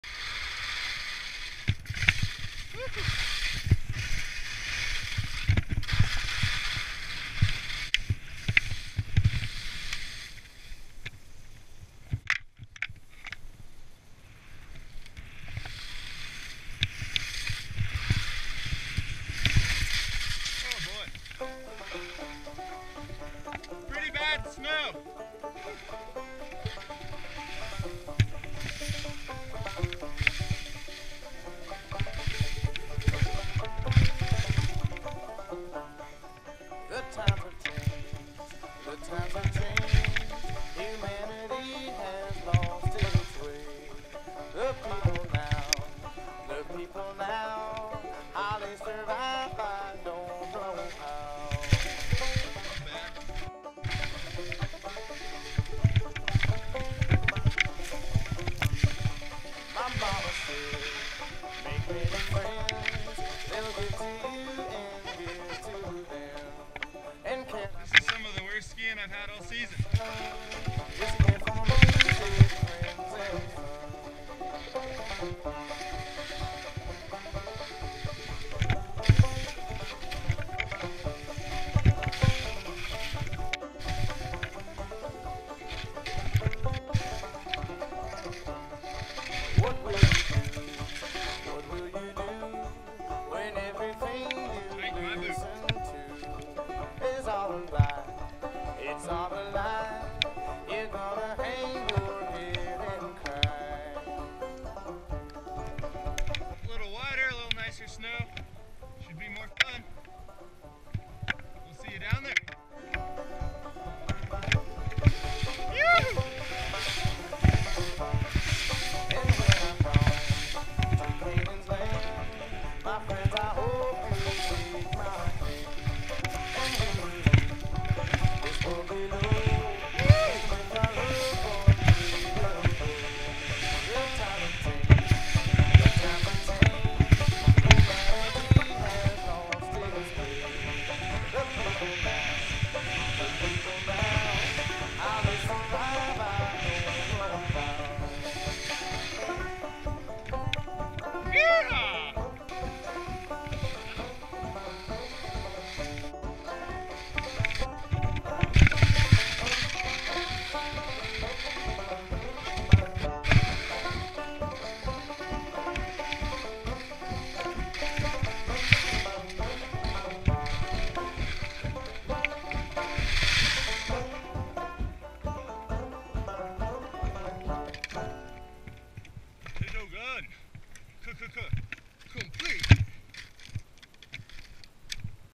Fun in the Potato Gun (Couloir)
Nothing like 500 feet of hop turns on frozen roller balls! Skiing the potato gun couloir in the San Juan mountains!